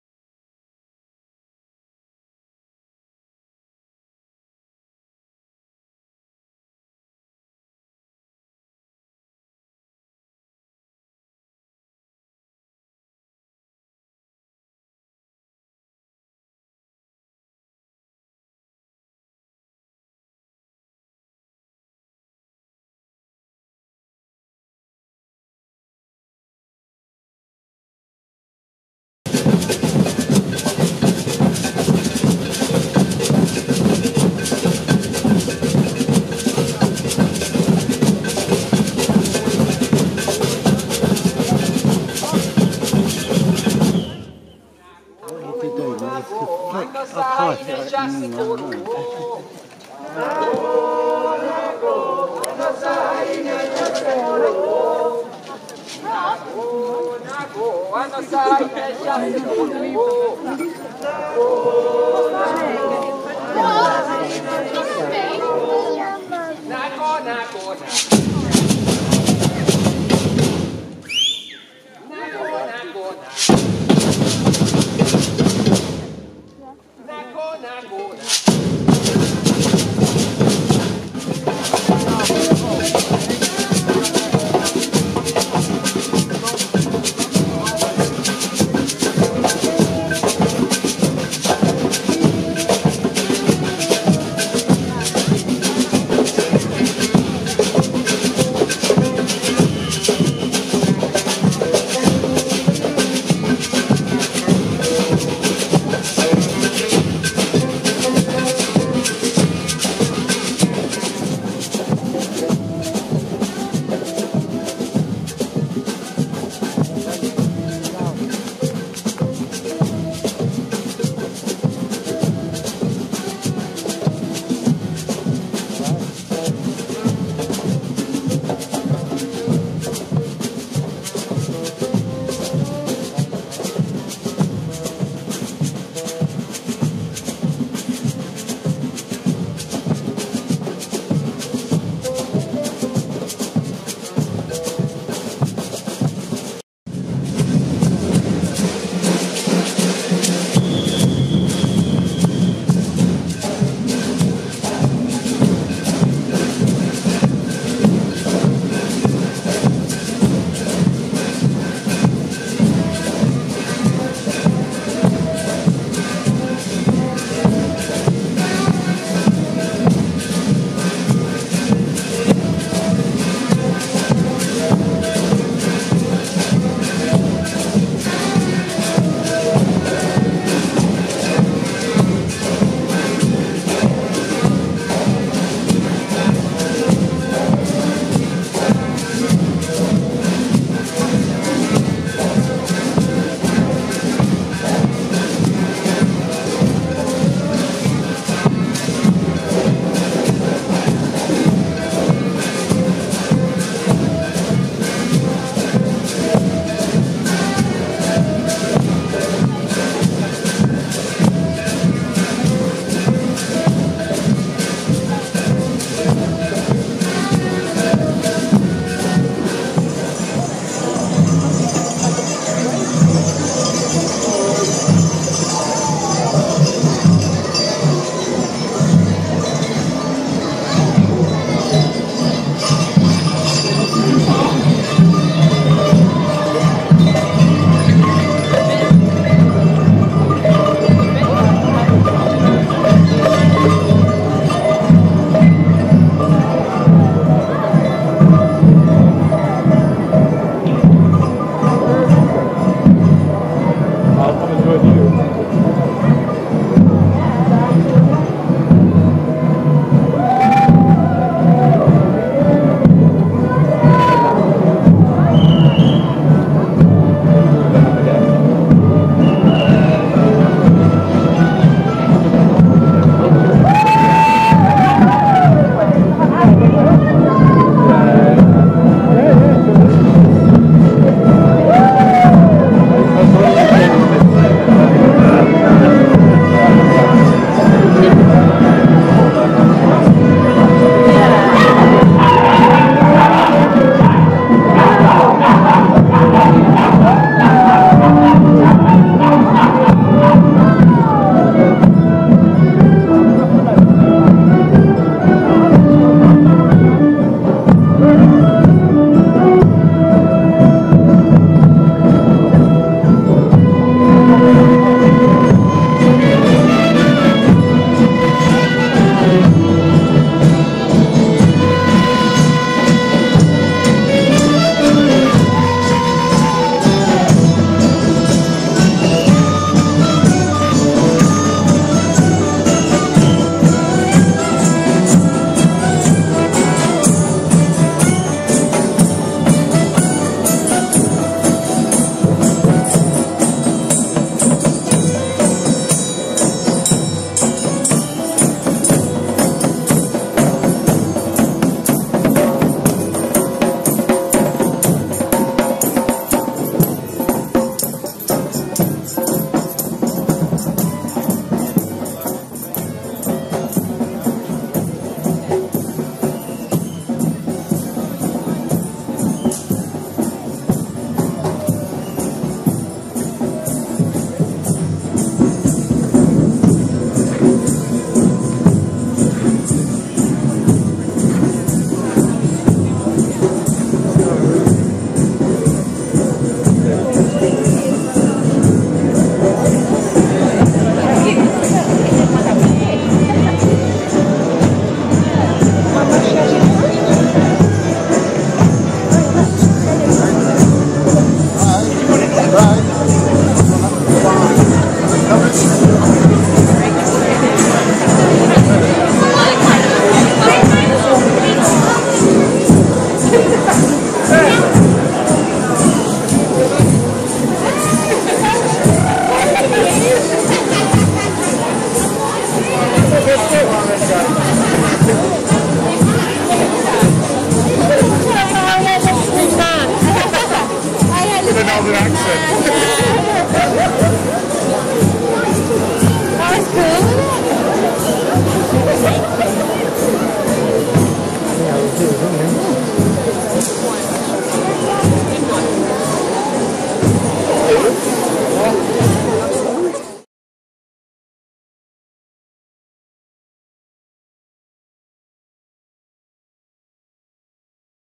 Montol 2015 - 6 o'clock Procession begins
A short film of the beginning of the evening's events in Penzance on 21 December 2016.  I don't know why the Penzance 'oss aka Penglaz, or Kasek-Nos, was absent this year?  Can anyone explain the unicorn carrying a silver teapot?